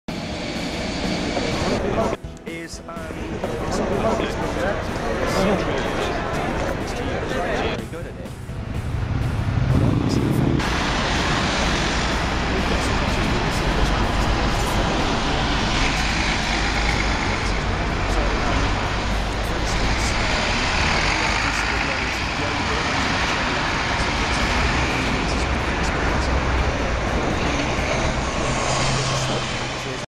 UK LONDON Global Style
The Global LONDON is leading in fashion, finance, Arts, commerce, education, tourism and transport. The traditional red double-decker buses keep the style and ride along the past and the future of the largest city in European Union. This HD video is Cultural to all ages. All the images are from Francisco Imperatore Fernandes. Enjoy. Thanks.